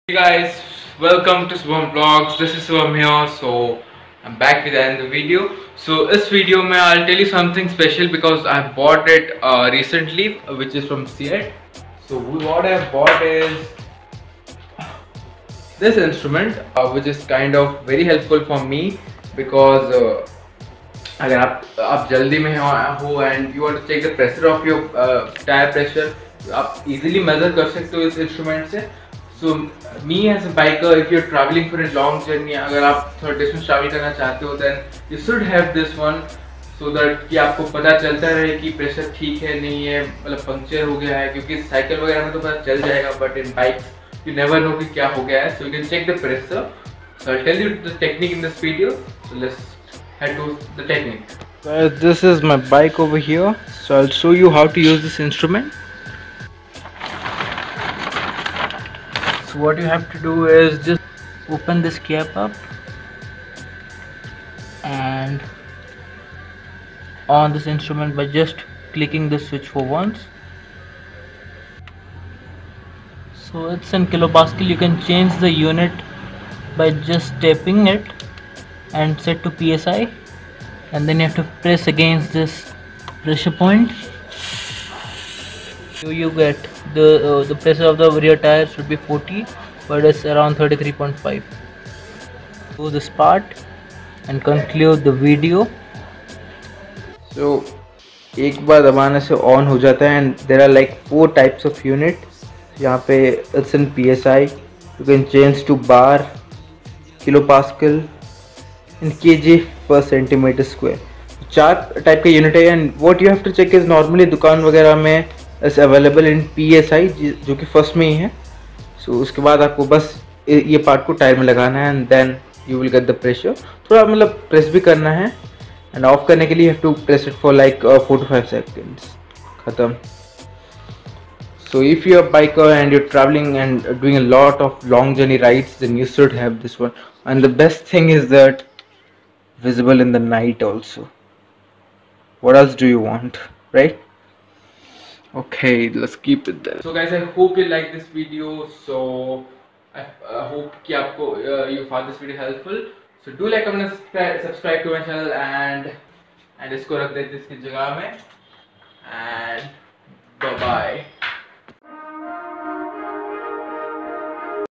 Digital Tyre Pressure Device For All Bikes
Hello guys in this video I have shown you people about the new instrument which I have bought for my bike,  which is very important for a tourer. Hope you guys find the video interesting and informative and nicely done.